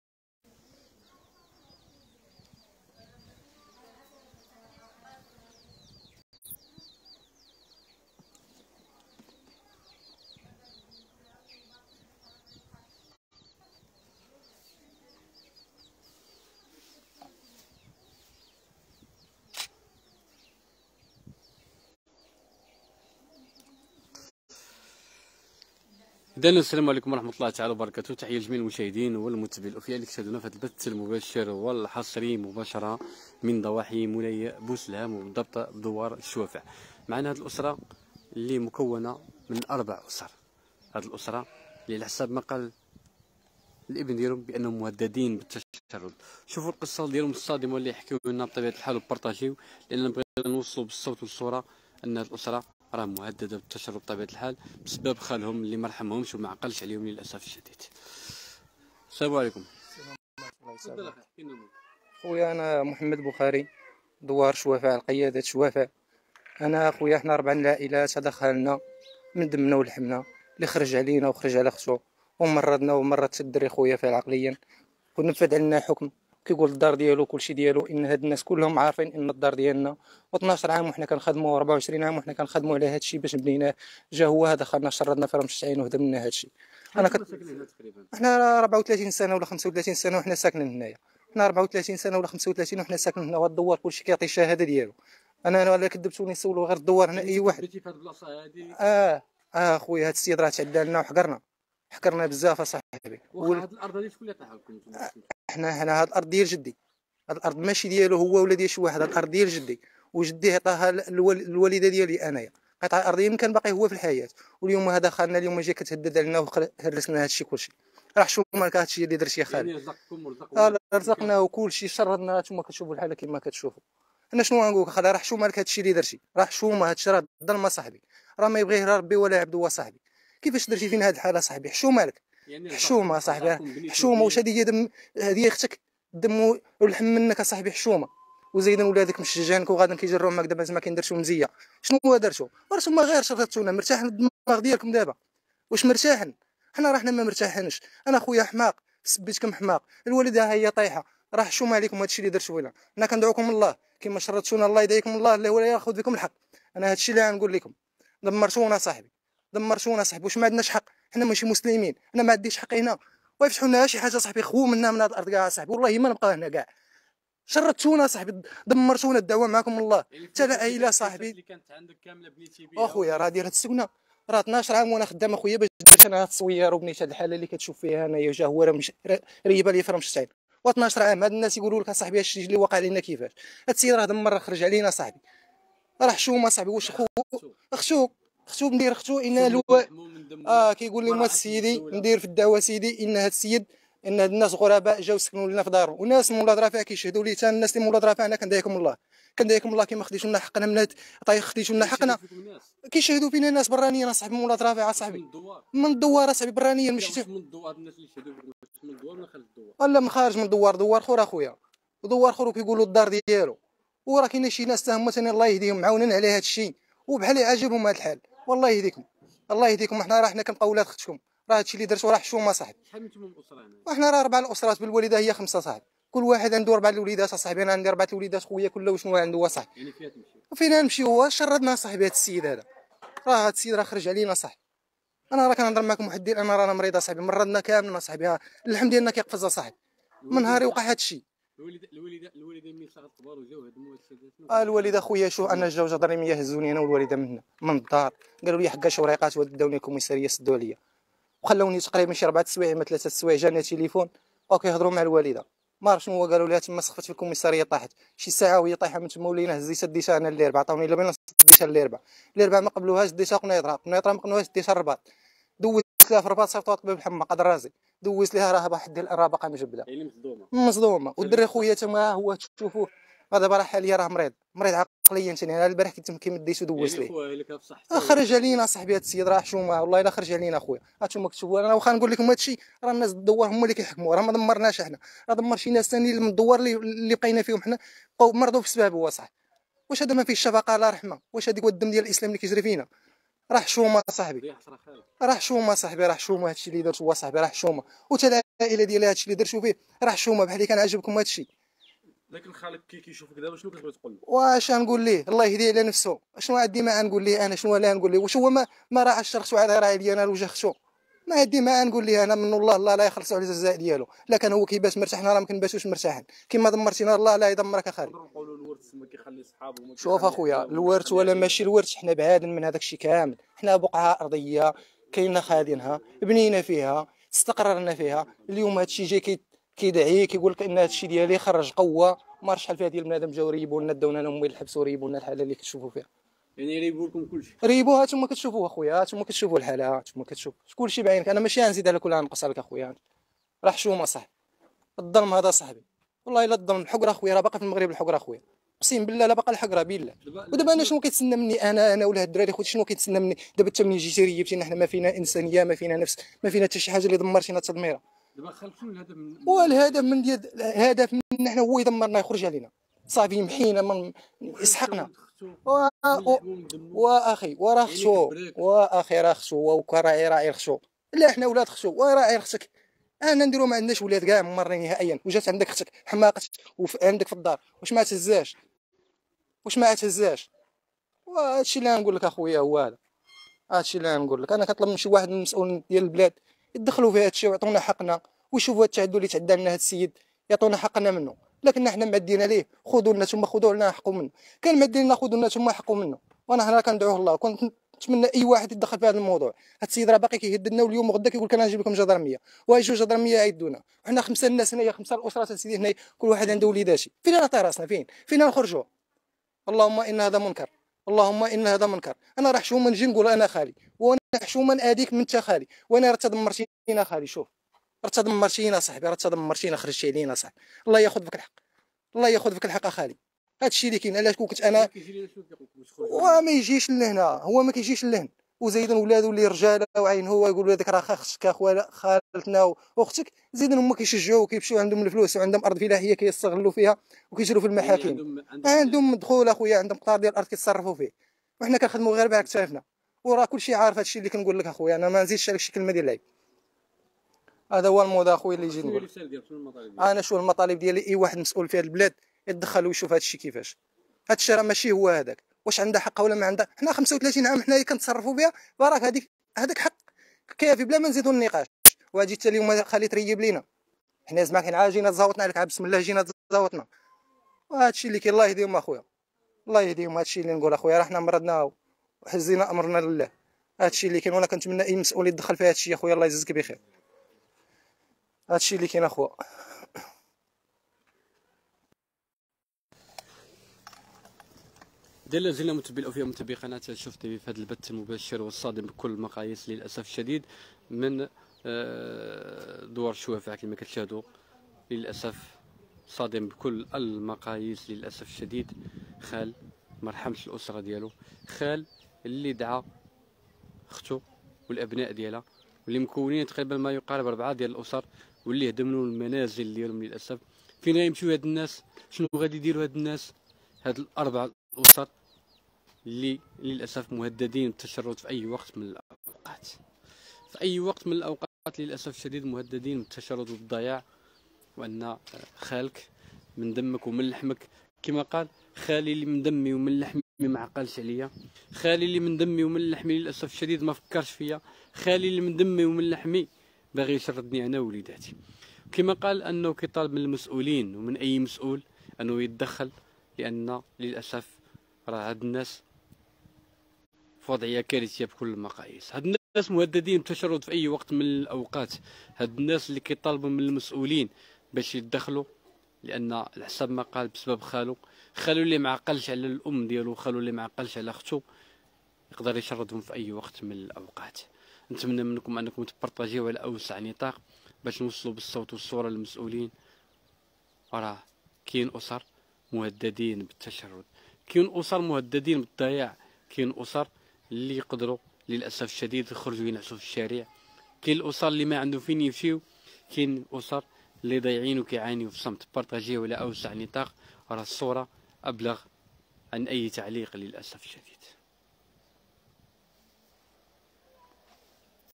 واش مبقاتش الرحمة؟ 4 أسر مهددين بالتشرد بسبب خالهم لي ريب ليهم ديورهم: "بغا ياخود لينا حقنا
مباشرة من نواحي مولاي بوسلهام: واش مبقاتش الرحمة؟ 4 أسر مهددين بالتشرد بسبب خالهم لي ريب ليهم ديورهم: "بغا ياخود لينا حقنا"